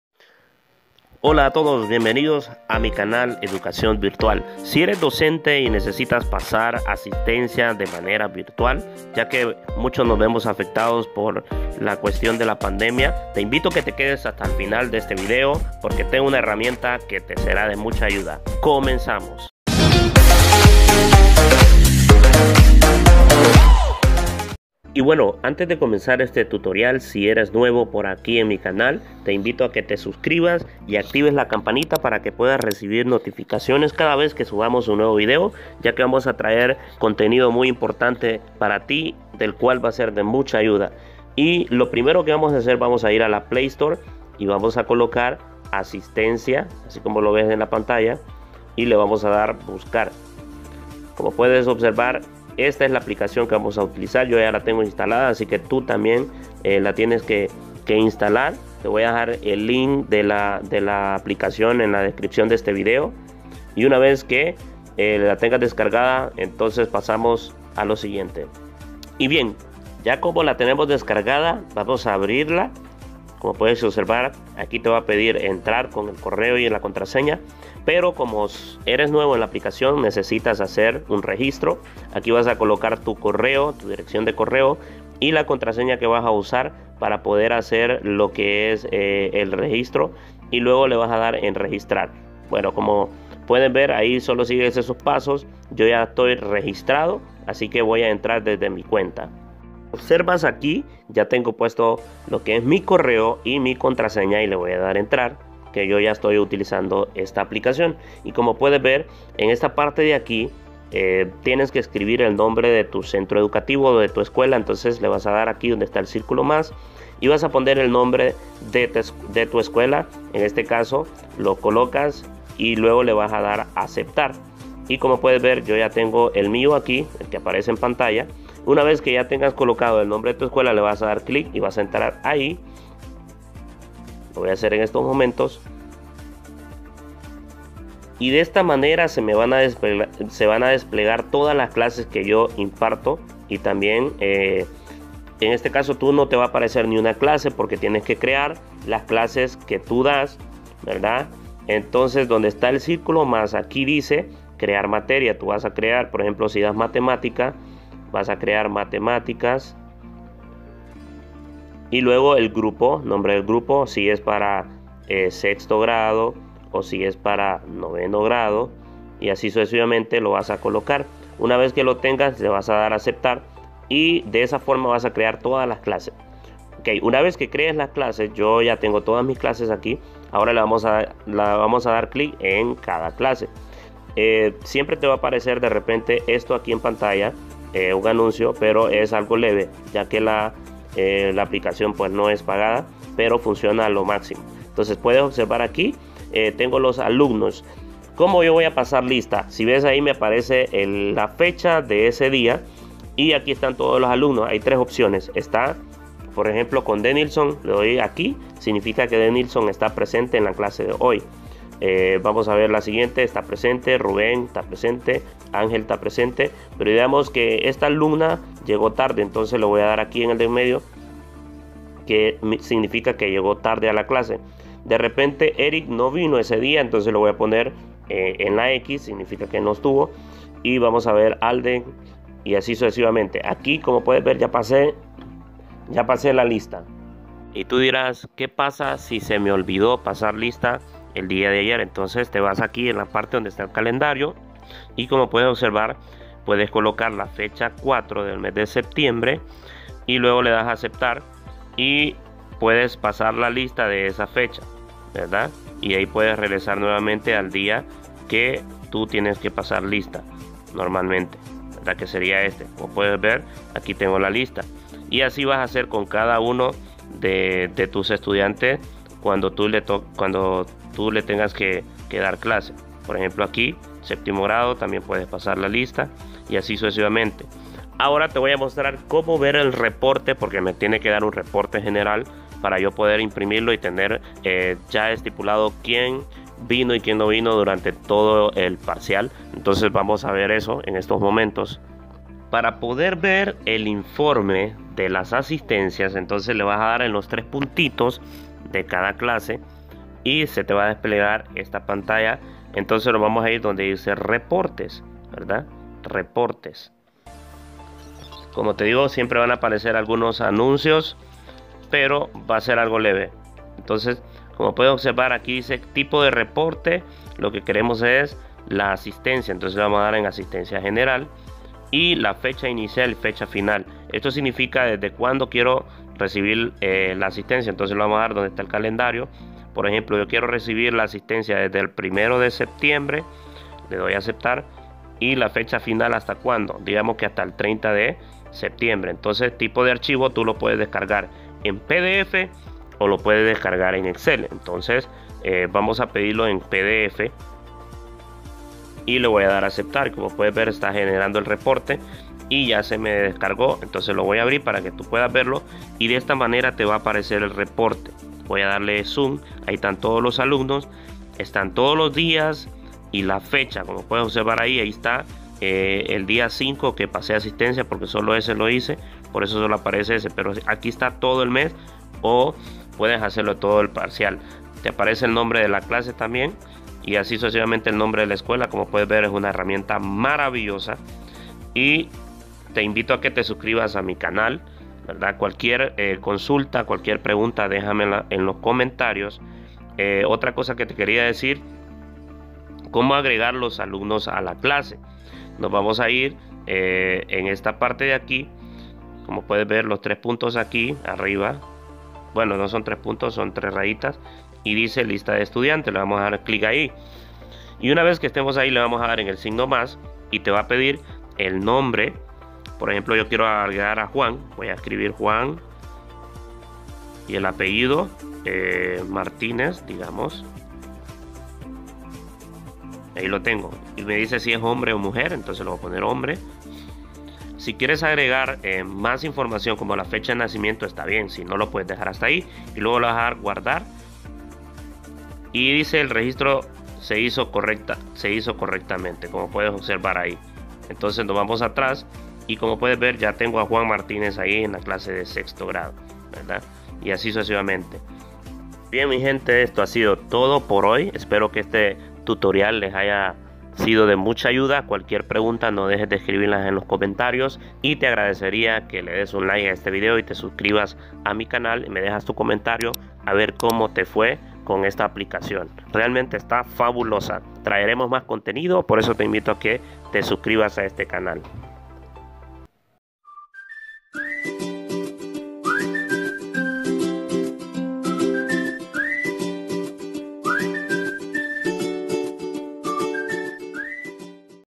CÓMO PASAR ASISTENCIA VIRTUAL 2020 📚📲📱
Hoy te comparto una aplicación en la que vas a poder pasar asistencia a tus alumnos de una manera muy fácil y rápida. Dejanos tú comentario si te funcionó o si tienes alguna duda me puedes escribir. 😊📚📚📚📚📱📱📲📲📲